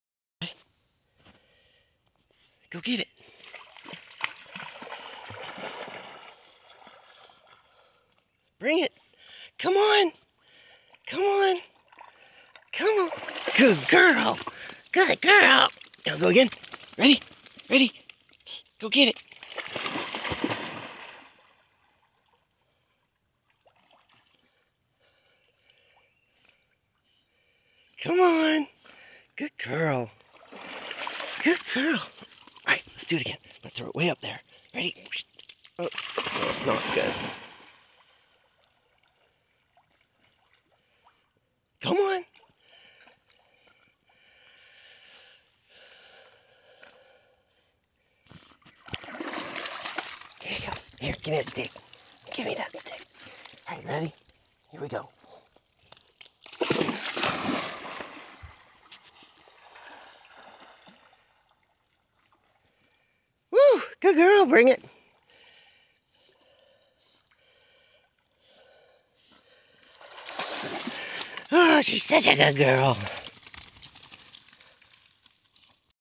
Ella Fetching Sticks In The River
dog, golden retriever, fetch, river